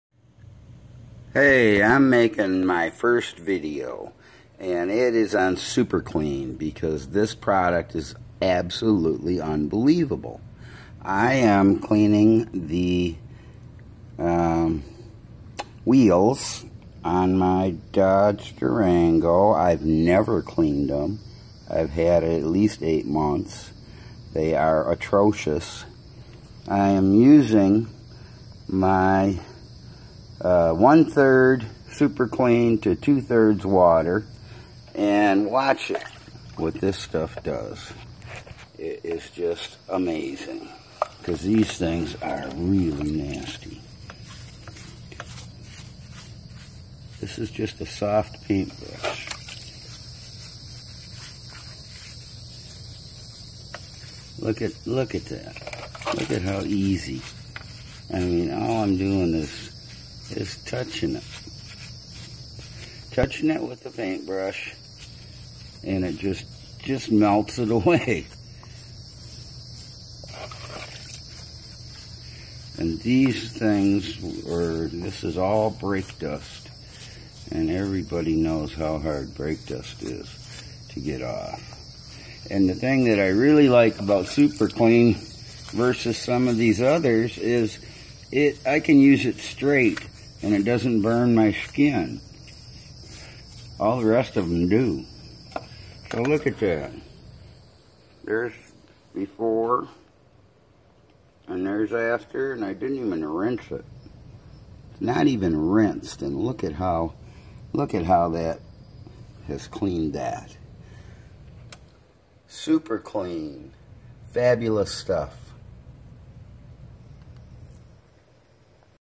Super clean
This is a video demonstrating how well super clean cleans